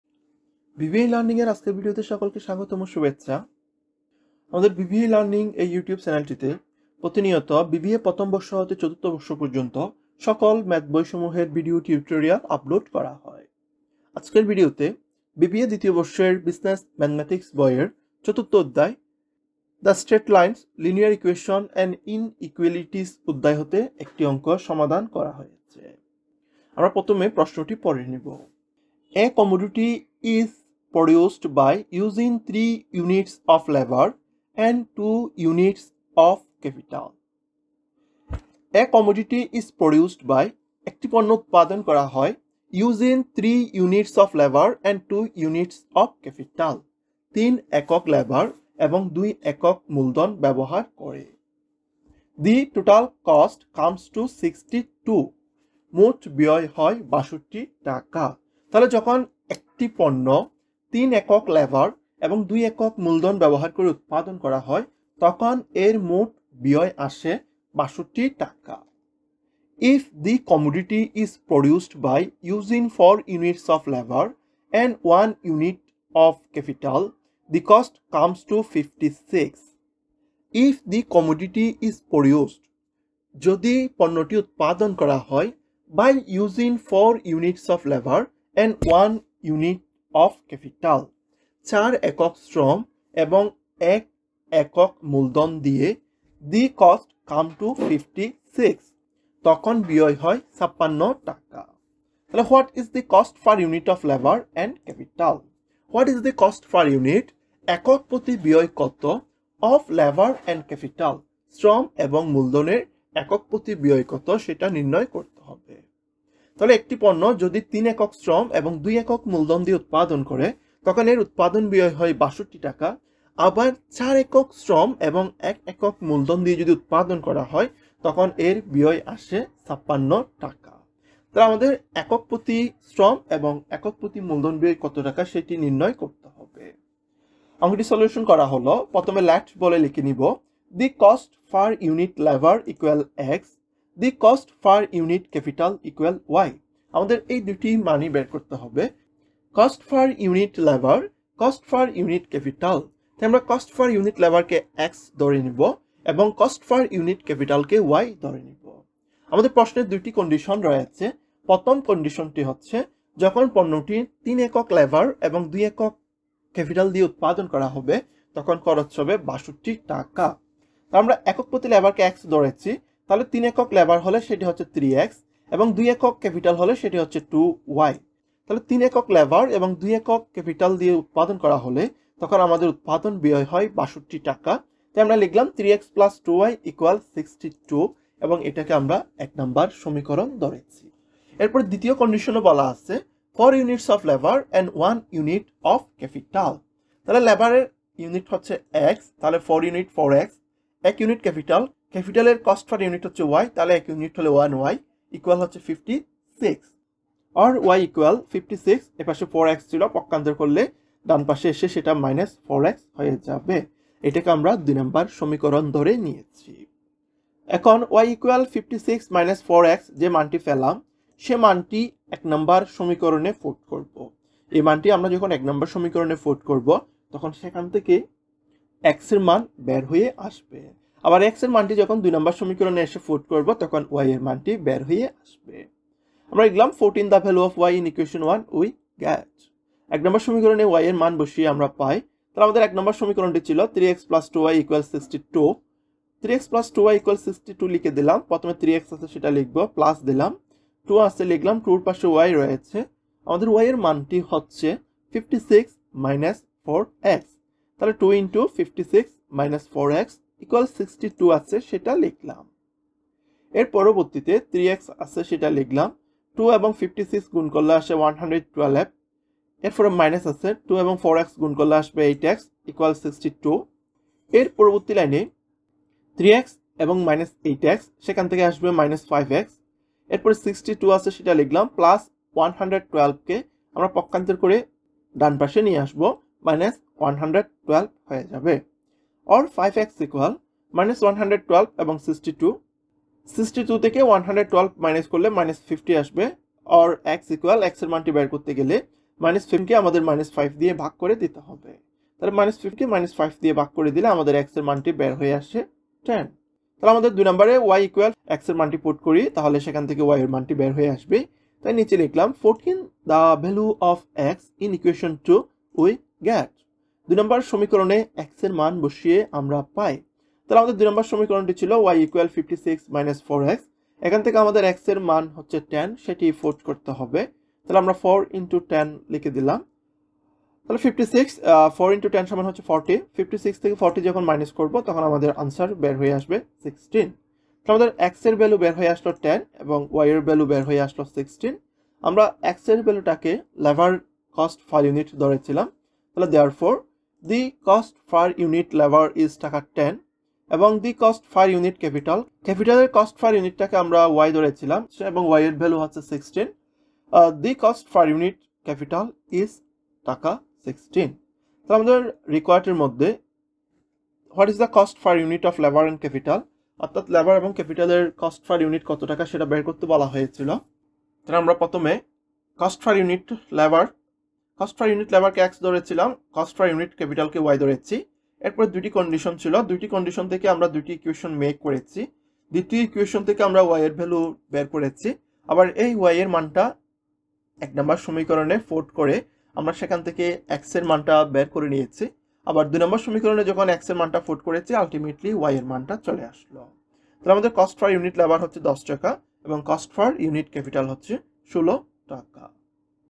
Business Mathematics Chapter: The Straight Lines, Linear Equations and Inequalities| Part 5
A National University math solution of Business Mathematics of the chapter 4 The Straight Lines, Linear Equations and Inequalities.
Find out the cost per unit of labour and capital.